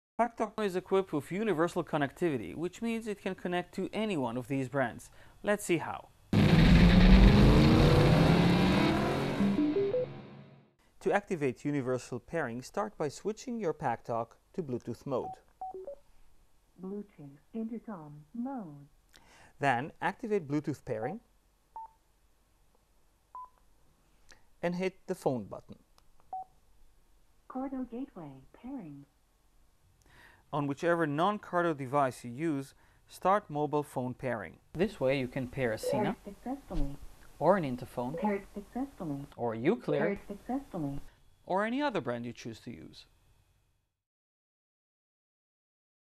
Универсальный канал Cardo GetAway на Packtalk для сопряжения с любой! гарнитурой
Универсальный канал Cardo GetAway на FREECOM для сопряжения с любой! гарнитурой. Как этим пользоваться и для чего оно нужно.